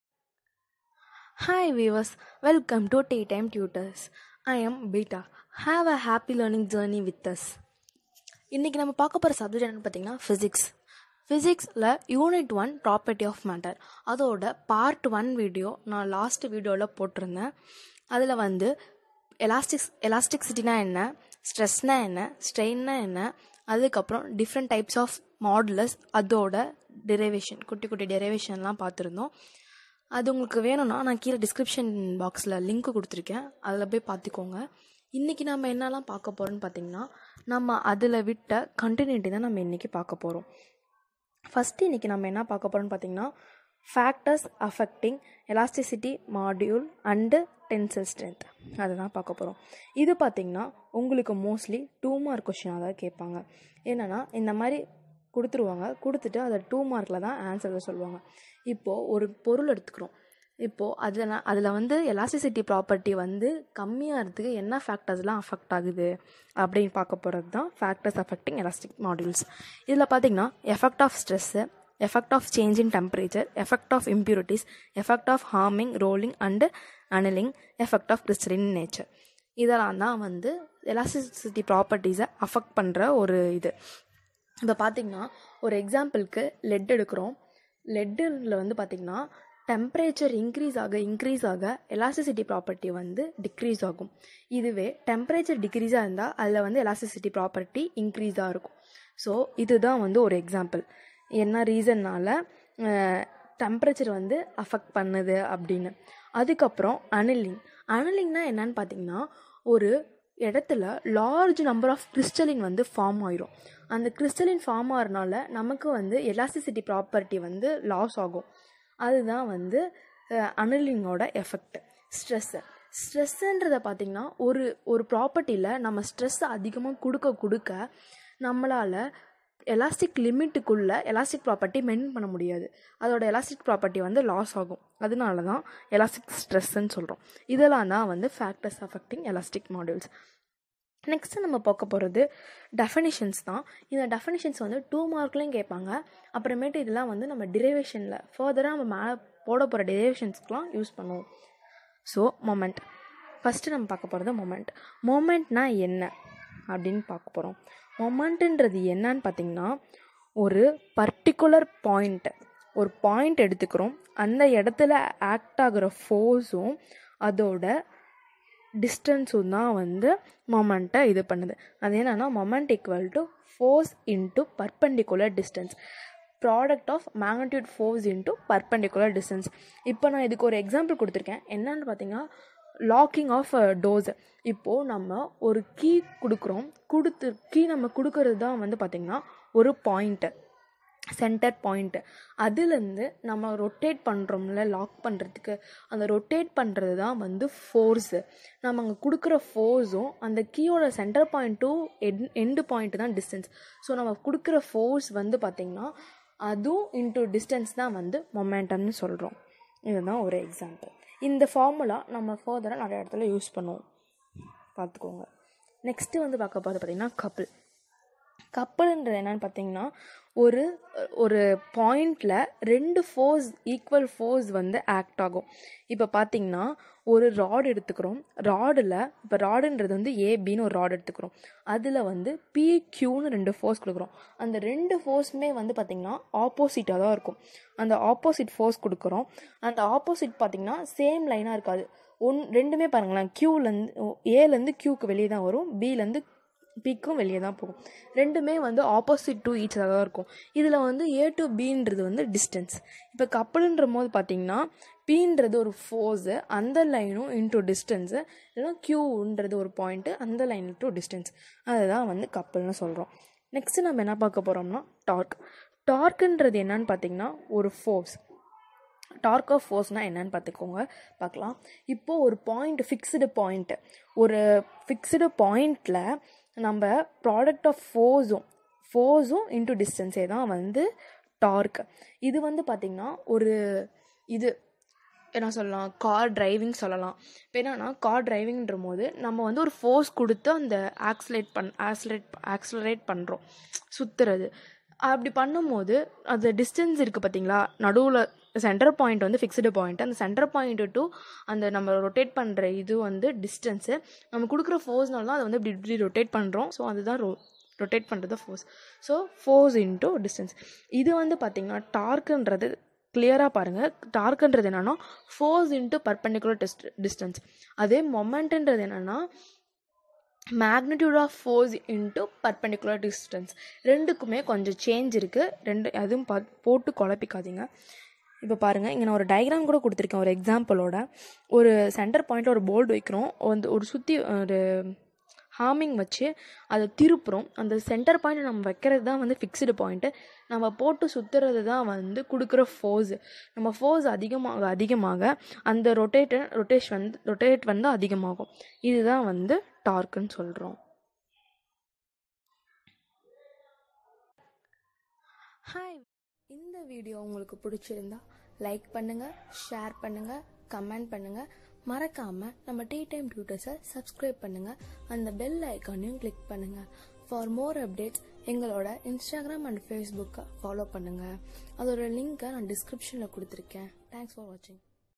Factors affecting Elastic modulus and tensile strength | Explained in Tamil | Engineering Physics
We are the students like you initiating our first step to share our pathway in this engineering field.In this video we shared about PH8151-Engineering Physics which comes in first semester and it is common to all branches under Anna University syllabus.Hope you learn from it.Happy learning!!!